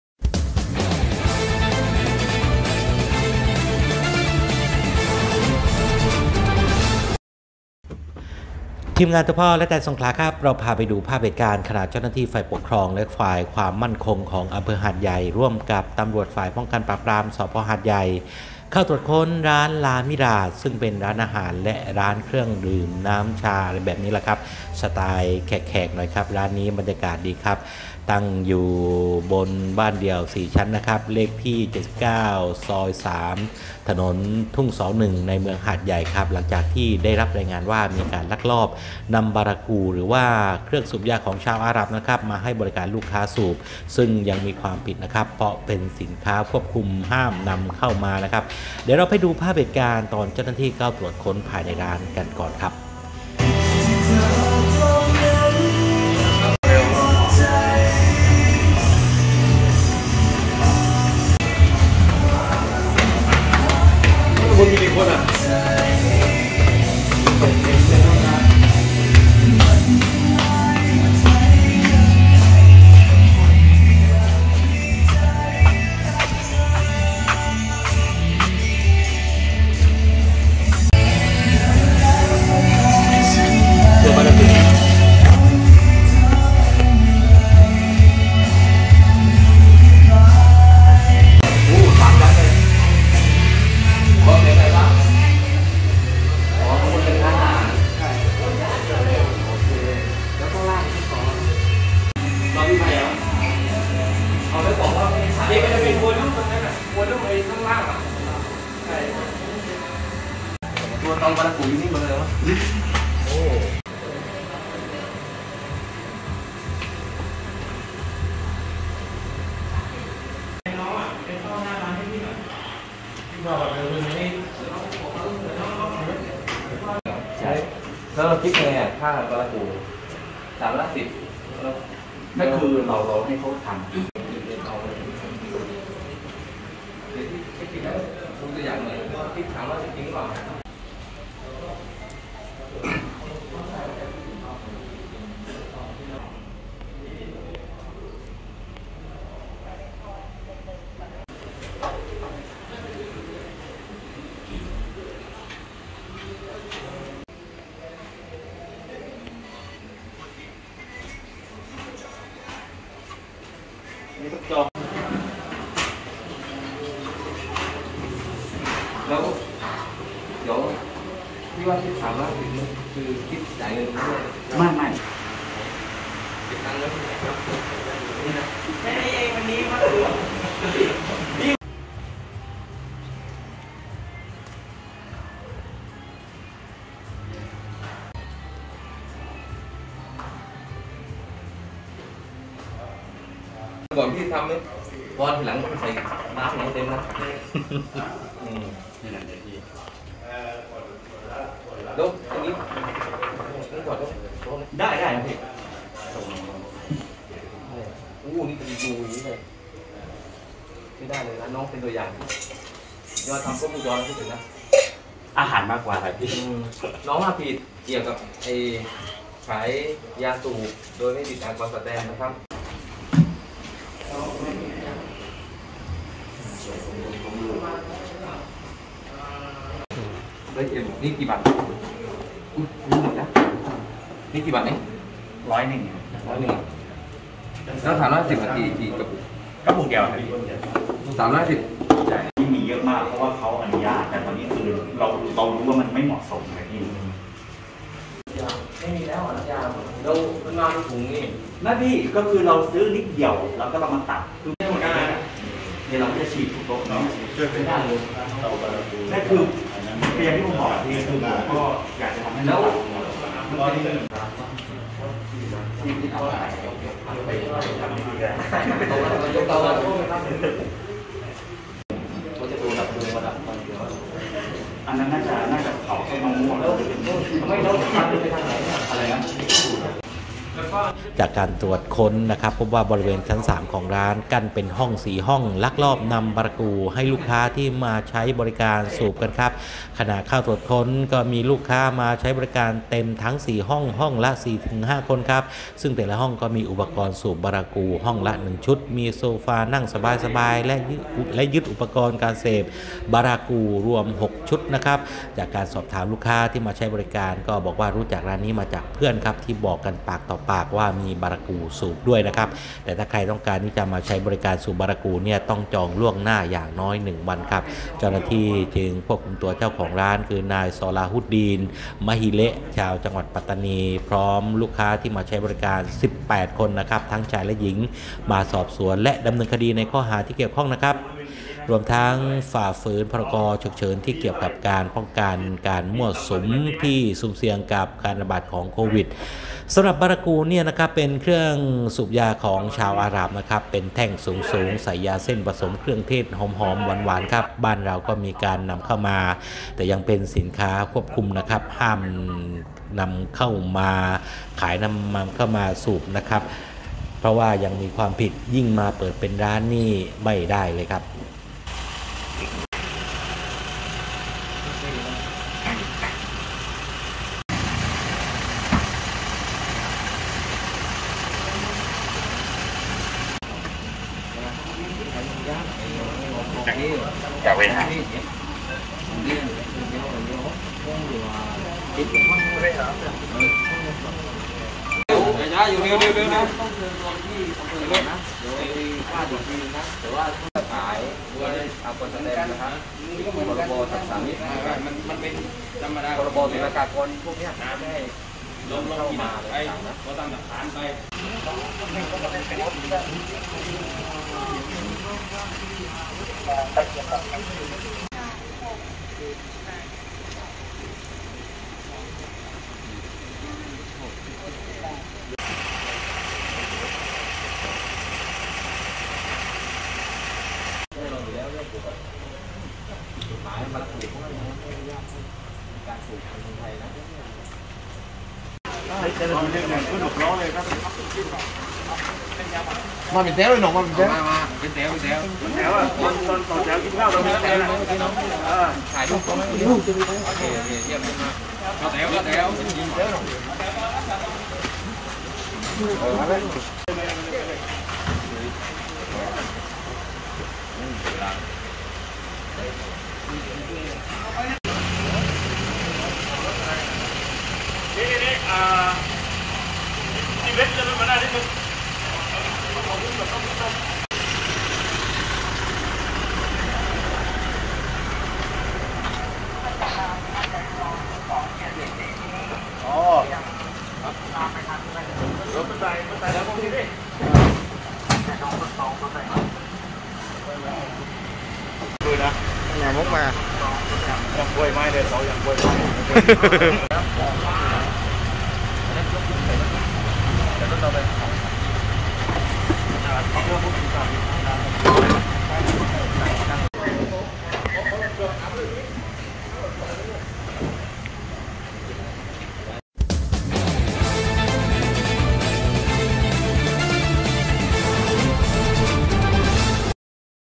@หาดใหญ่จบทั้งร้าน!!บารากู่!!หอมหวานเกินห้ามใจ!!
หาดใหญ่-เจ้าหน้าที่ฝ่ายปกครองและฝ่ายความมั่นคงอำเภอหาดใหญ่และตำรวจสภ.หาดใหญ่ ตรวจค้นร้าน ลา มิราจ ซึ่งเป็นร้านอาหารและร้านเครื่องดื่มน้ำย่านทุ่งเสา ในเมืองหาดใหญ่ ลักลอบขายบารากู่ให้ลูกค้าสูบรวบทั้งเจ้าของและนักเที่ยว18คน เมื่อช่วงดึกเมื่อคืนนี้(7ม.ค.65)

Hat Yai - Administrative and security officials in Hat Yai District and Hat Yai Police Station searched La Mirage, a restaurant and beverage shop in Thung Sao area. in Hat Yai Smuggling hookahs to customers, collecting both owners and 18 tourists Late last night (8 Jan. 65)